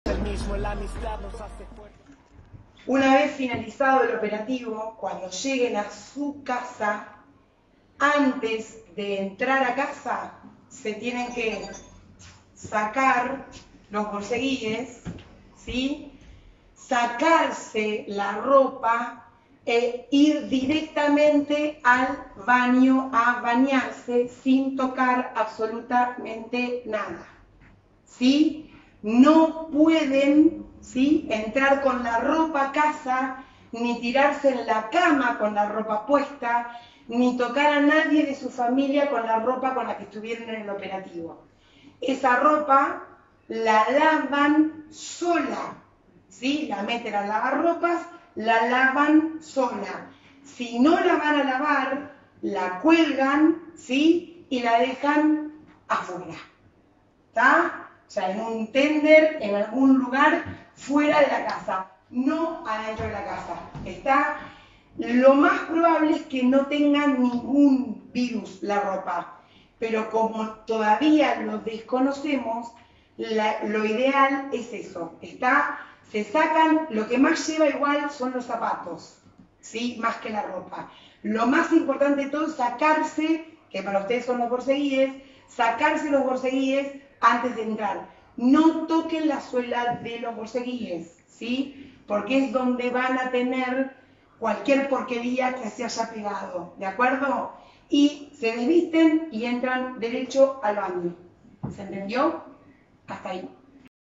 PANDEMIA: ¿Que debe hacer un policía cuando llega a su casa luego de trabajar?
Desde UTRAPOL y APROPOL compartimos con ustedes un video de instrucción de la Policía Bonaerense a todos los efectivos para que apliquen mínimas pero importantes medidas de bio-seguridad al llegar a sus domicilios.
Por Rubén Pombo

Básicamente la idea es tomar conciencia de que debemos extremar las medidas en todo momento pero con especiales características al llegar a nuestros hogares donde ademas esta nuestra familia y cuyo procedimiento es diferente en parte a los que realizamos durante el servicio.

Resumen:

Nos descalzamos (el calzado, zapatos, borceguies, etc) quedan fuera de la casa.

La ropa no ingresa al domicilio, se descambian y va SOLA al lavado (no mezclar con otras prendas). Si no se lava se cuelga en el exterior. NUNCA INGRESA.

El personal va directamente al baño y se ducha con abundante jabón (recordemos que este producto es el mas económico y eficaz bien aplicado es el mejor aliado contra el virus).

RECALCA SACARSE EL CALZADO ANTES DE ENTRAR. NUNCA ENTRAN AL INTERIOR DE LA CASA. ALLÍ SE ALOJAN POSIBLEMENTE TODOS LOS VIRUS QUE ENCUENTREN DURANTE EL DÍA.

APROPOL Noticias / UTRAPOL Noticias